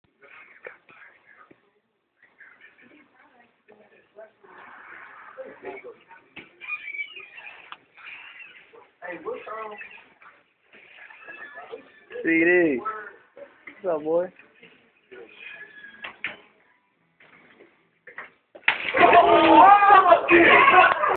Old Man Vs. Cherry Pie
So my old manager was an asshole, and nobody liked him. But nobody had the balls to do anything. well until i decided to put it into my own hands..